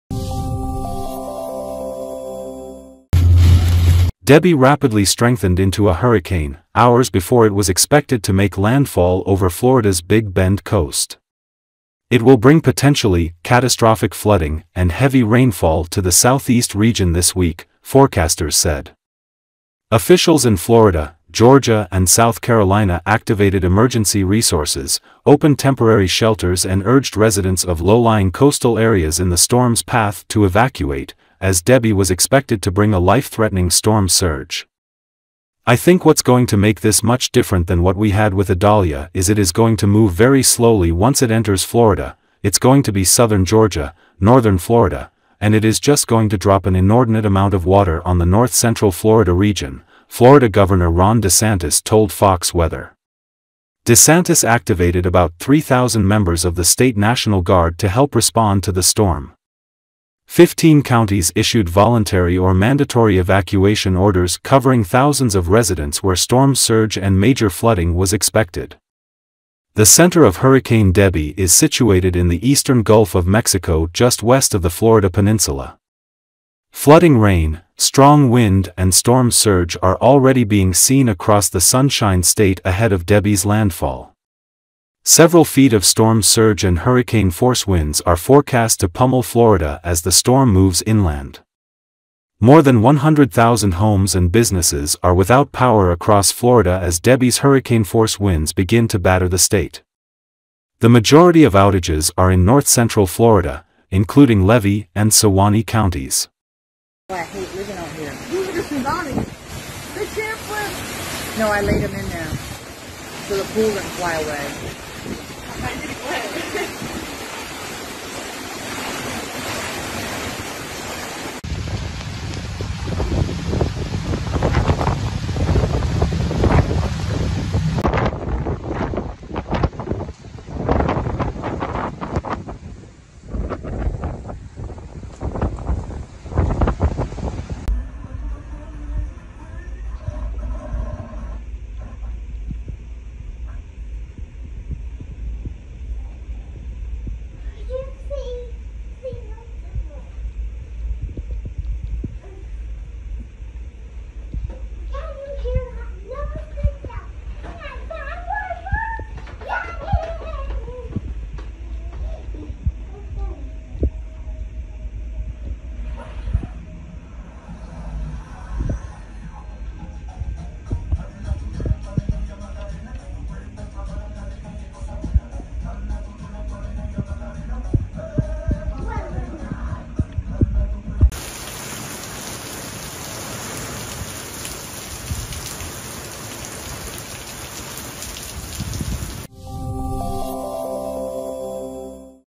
Debby strengthens to a hurricane as it pummels Florida with flooding rain
Debby rapidly strengthened into a hurricane, hours before it was expected to make landfall over Florida’s Big Bend coast. It will bring potentially “catastrophic flooding” and heavy rainfall to the Southeast region this week, forecasters said.

Officials in Florida, Georgia and South Carolina activated emergency resources, opened temporary shelters and urged residents of low-lying coastal areas in the storm’s path to evacuate, as Debby was expected to bring a life-threatening storm surge.

"I think what’s going to make this much different than what we had with Idalia is it is going to move very slowly once it enters Florida – it’s going to be southern Georgia, northern Florida – and it is just going to drop an inordinate amount of water on the north central Florida region," Florida Governor Ron DeSantis told FOX Weather. DeSantis activated about 3,000 members of the state National Guard to help respond to the storm. Fifteen counties issued voluntary or mandatory evacuation orders covering thousands of residents where storm surge and major flooding was expected.

The center of Hurricane Debby is situated in the eastern Gulf of Mexico just west of the Florida Peninsula.

Flooding rain, strong wind and storm surge are already being seen across the Sunshine State ahead of Debby's landfall. Several feet of storm surge and hurricane force-winds are forecast to pummel Florida as the storm moves inland.

More than 100,000 homes and businesses are without power across Florida as Debby’s hurricane-force winds begin to batter the state.

The majority of outages are in north-central Florida, including Levy and Suwannee counties.